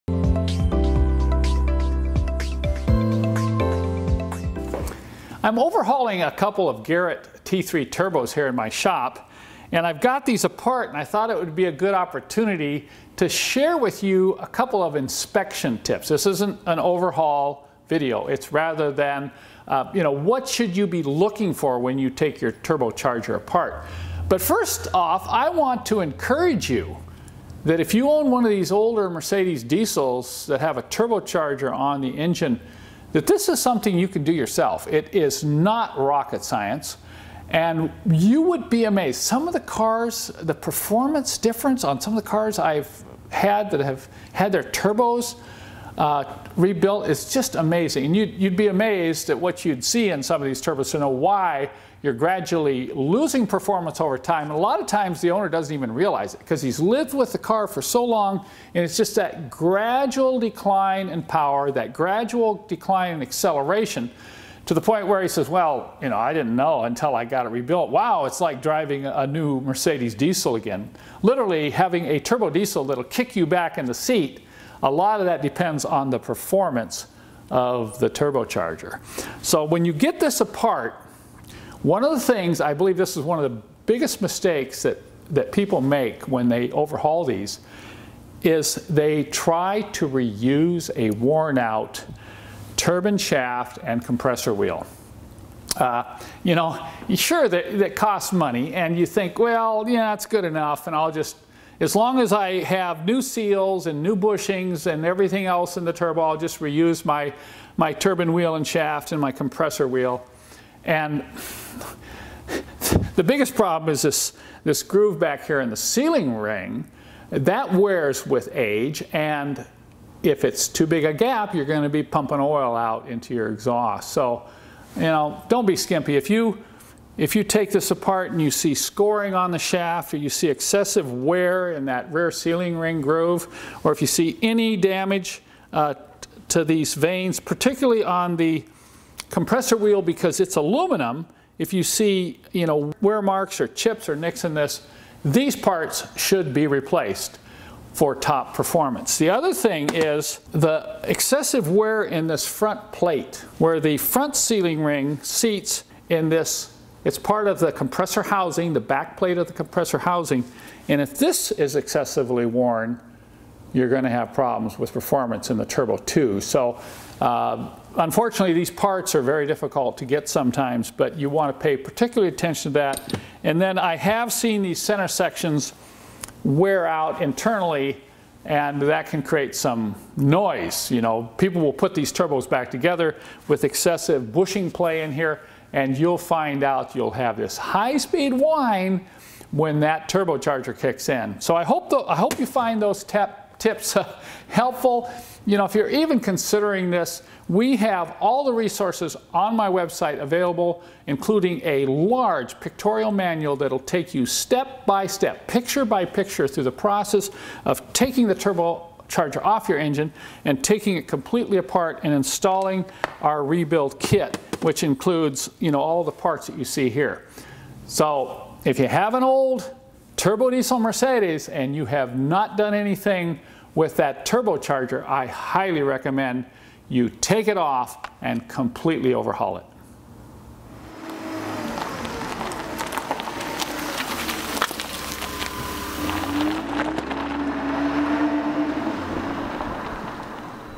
Mercedes Garrett T3 Turbo Charger Internal Inspection
Kent goes over some things you should pay close attention to when opening up your Garrett T3 turbocharger for overhaul. There are some parts that often do not get replaced that should.